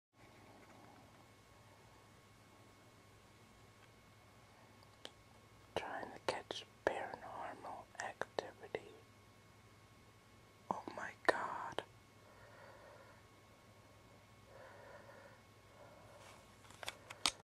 spirit light in my room
I was just messing around and then I saw the light. I was so scared I had to turn it off